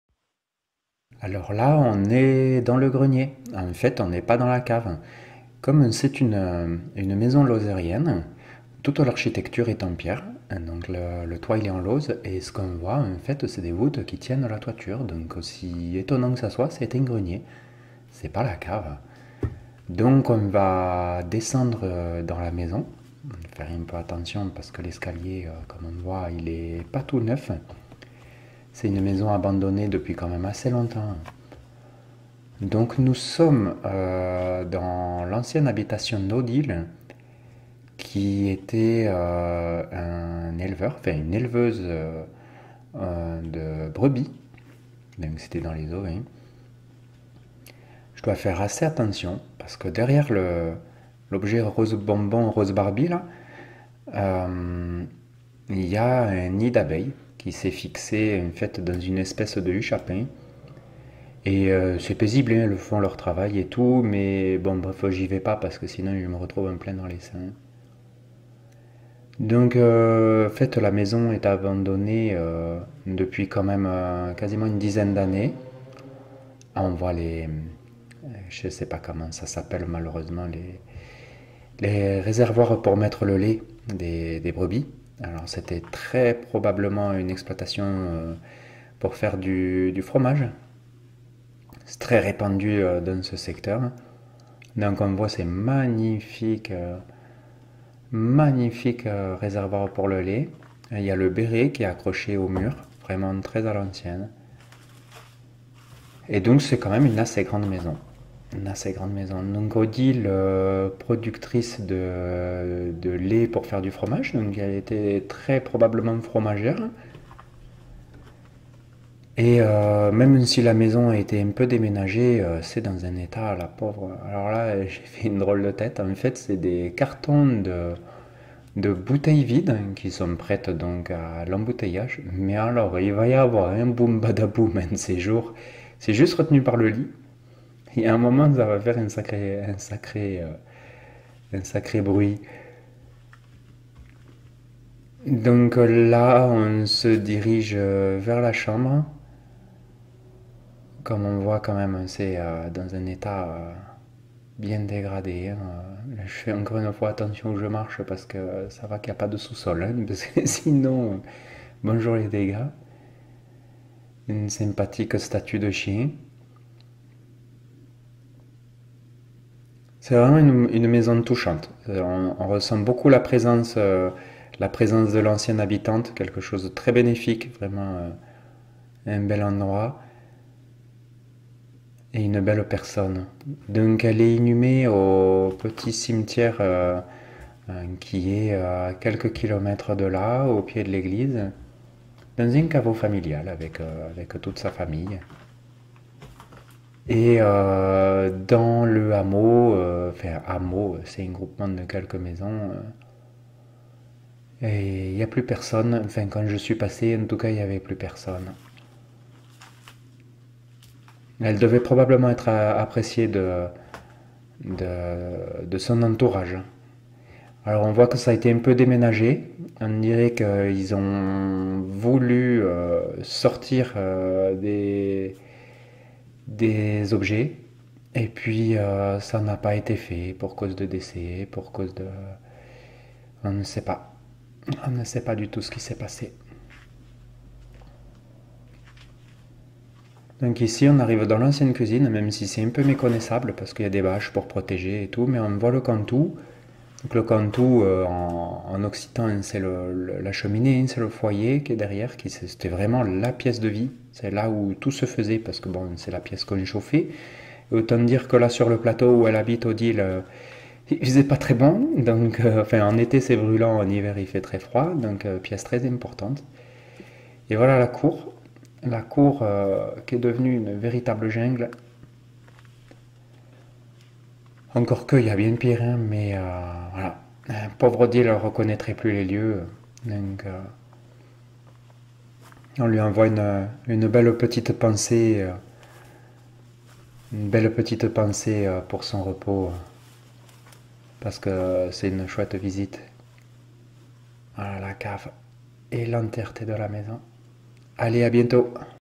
🚷 URBEX - LA MAISON ODILE
Visite de la maison Odile, une ferme isolée sur les hauts plateaux. On trouve des traces touchantes de vie, avec des vestiges d'élevage de brebis. C'est une maison très touchante.